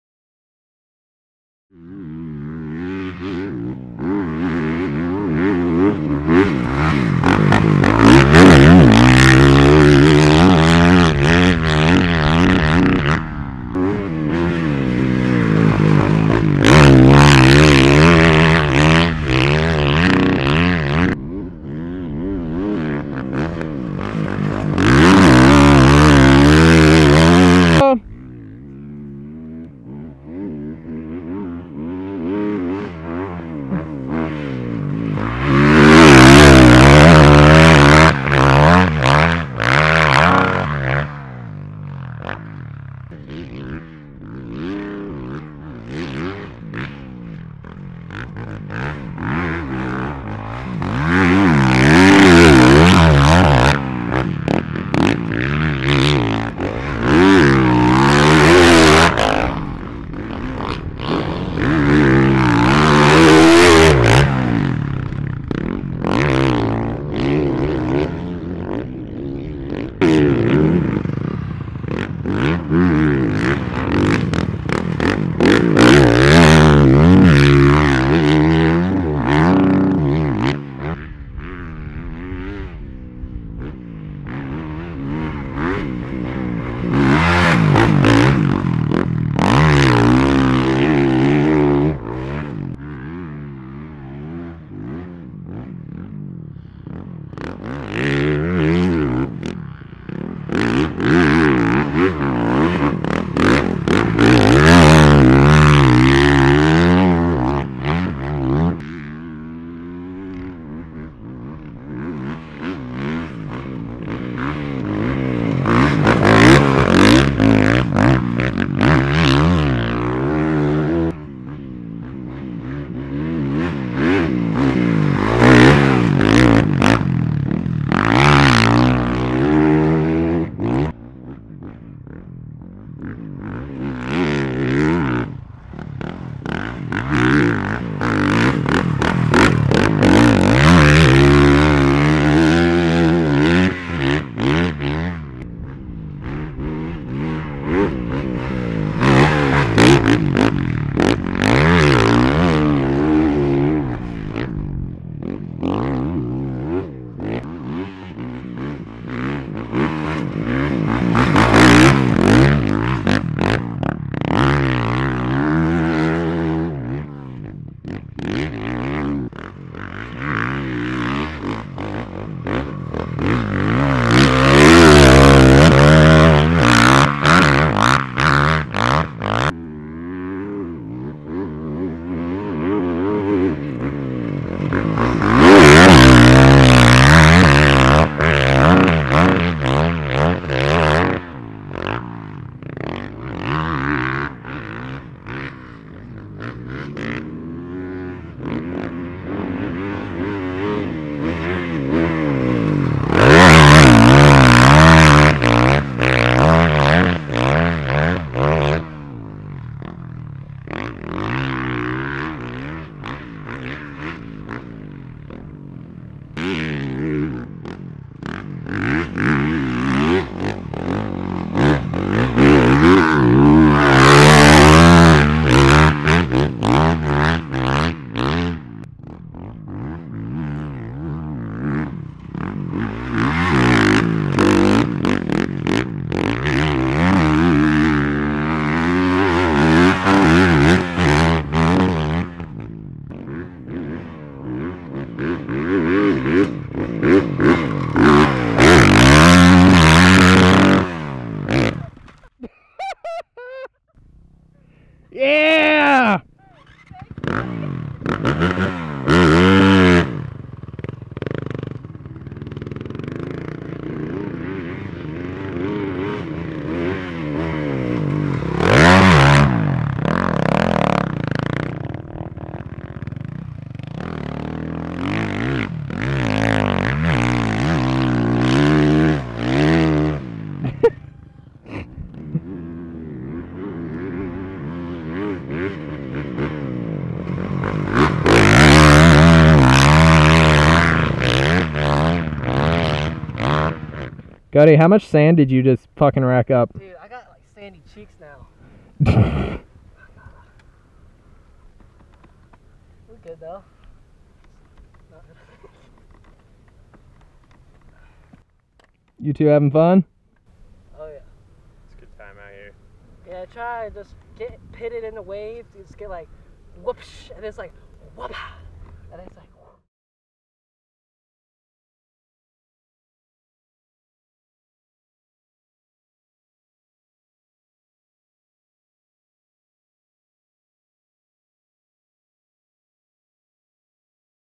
RAW: Jeff Walker and Coty Schock - BFE Sand Track
With the down time of the Supercross season being cancelled, it's time to hit some outdoor training with FXR/Chaparral Honda's Jeff Walker and Coty Schock. And with all the rain in So-Cal, it's time to find some new places to ride...